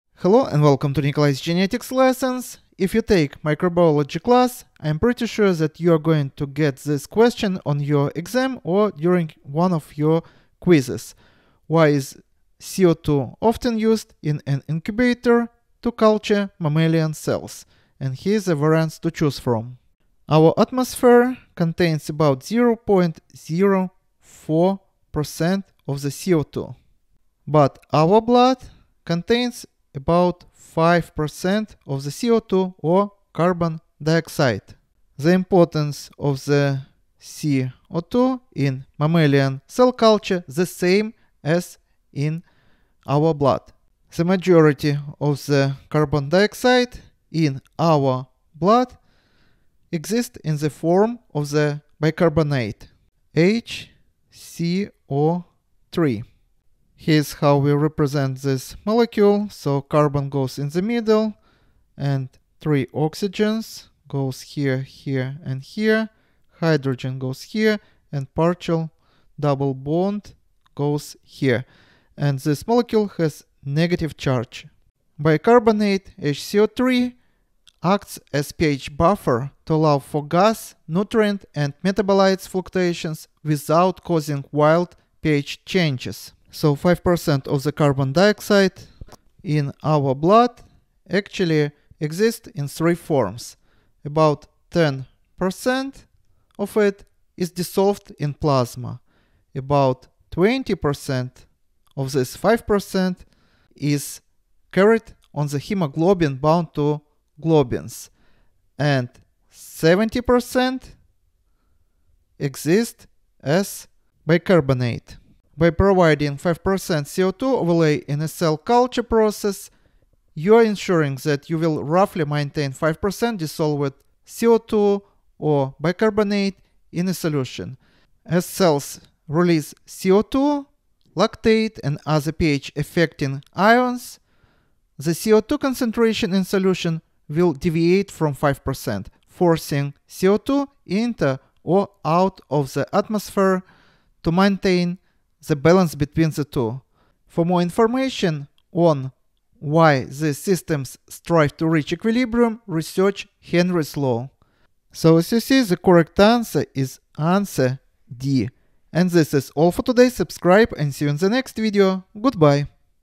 The Crucial Role of CO2 in Mammalian Cell Culture
In the realm of mammalian cell culture, maintaining an optimal pH is of paramount importance for the growth and viability of cells. The delicate balance of pH is crucial for cellular processes, enzymatic activity, and overall cell health. While various factors contribute to pH regulation in cell culture, the presence of carbon dioxide (CO2) plays a central role. This article explores the significance of CO2 in mammalian cell culture for pH regulation and highlights its importance in achieving successful cell growth and experimental outcomes.

The Role of pH in Cell Culture:

pH, which measures the acidity or alkalinity of a solution, is a fundamental parameter in cell culture. Mammalian cells thrive within a narrow pH range, typically around 7.2 to 7.4, which closely resembles the physiological conditions found in the human body. Maintaining this optimal pH range is essential for proper cell functioning, enzymatic activity, protein synthesis, and maintaining cellular homeostasis.

CO2 and pH Regulation:

Carbon dioxide (CO2) is a critical component in regulating pH levels in mammalian cell culture systems. When CO2 is dissolved in water, it reacts with water molecules to form carbonic acid (H2CO3), a weak acid. This reaction is catalyzed by the enzyme carbonic anhydrase, which is present in mammalian cells. Carbonic acid then dissociates into bicarbonate ions (HCO3-) and hydrogen ions (H+). The release of hydrogen ions helps to maintain the appropriate pH balance.

In cell culture, CO2 is introduced into the incubator environment. It equilibrates with the culture medium, leading to the formation of carbonic acid. The carbonic acid, in turn, acts as a buffering system, helping to stabilize the pH of the culture medium. This buffering capacity ensures that even slight changes in pH are counteracted, maintaining the desired pH range for optimal cell growth.

Importance in Cellular Metabolism:
CO2 also plays a vital role in cellular metabolism, which influences pH regulation in cell culture. During cellular respiration, cells produce CO2 as a byproduct. This CO2 is released into the culture medium, contributing to the buffering capacity and aiding in pH regulation. As cells consume oxygen and generate CO2, the equilibrium of the carbonic acid-bicarbonate system is maintained, stabilizing the pH within the desired range.

Controlled Atmosphere:

The presence of CO2 in the incubator creates a controlled atmosphere that mimics physiological conditions for mammalian cells. By regulating CO2 concentration, the pH of the culture medium can be precisely maintained, providing an optimal environment for cell growth and proliferation.

Conclusion:

The importance of CO2 in mammalian cell culture for pH regulation cannot be overstated. CO2 acts as a critical component in the buffering system, maintaining the optimal pH range required for cell growth and functionality. Through its role in the carbonic acid-bicarbonate equilibrium, CO2 ensures that the pH of the culture medium remains stable, providing a suitable environment for cell metabolism and enzymatic activity.

By understanding the significance of CO2 in pH regulation, researchers and scientists can create precise and controlled conditions for mammalian cell culture experiments. This knowledge is vital for achieving successful cell growth, maintaining cellular homeostasis, and obtaining accurate and reliable results in various fields, including biotechnology, pharmaceutical research, and regenerative medicine. The incorporation of CO2 in cell culture systems enhances our ability to study and manipulate mammalian cells, paving the way for advancements in biomedical research and therapeutic applications.

Problem:

Why is CO2 often used in an incubator to culture mammalian cells?

A) to control oxygen consumption
B) to control cell growth
C) to control or prevent viral/bacterial contamination
D) to control the pH